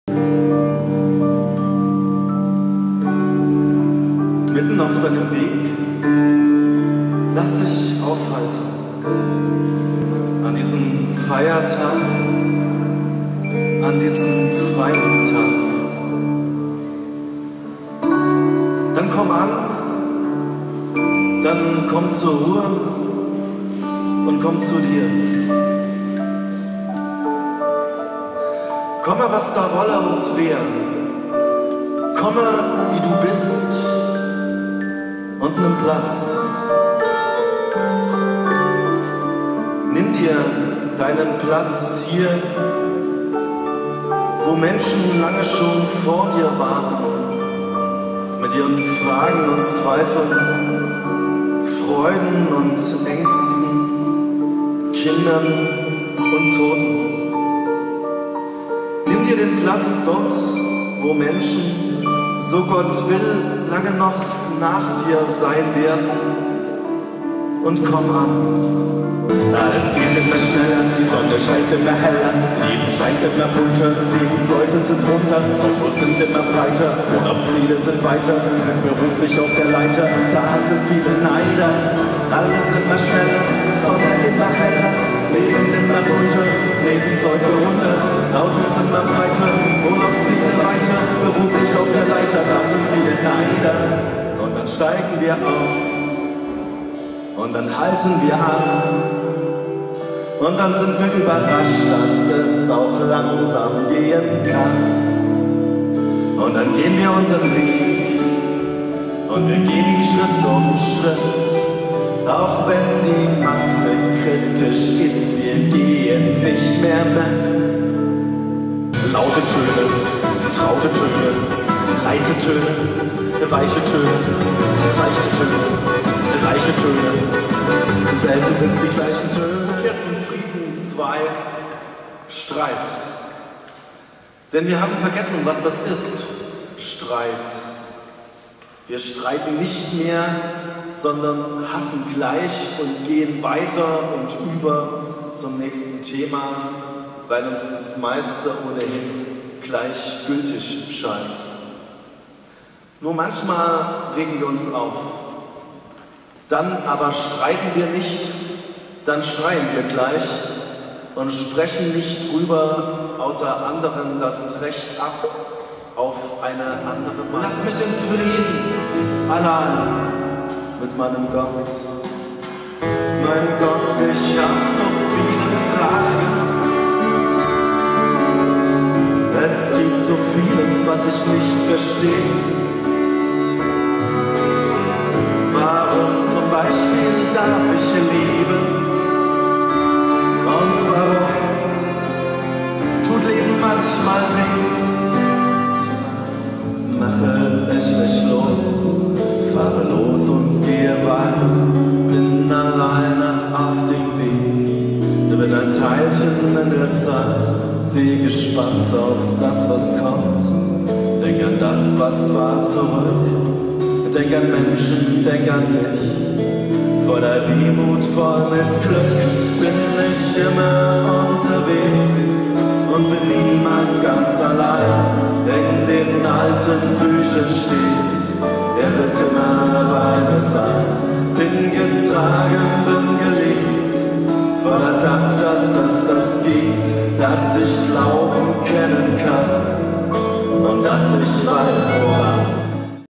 zwischenFall zum Friedensgottesdienst, Nikolaikirche Leipzig
Ein Zusammenschnitt des Abschlussgottesdienstes der Ökumenischen Friedensdekade zum Buß- und Bettag 2020 in der Nikolaikirche zu Leipzig.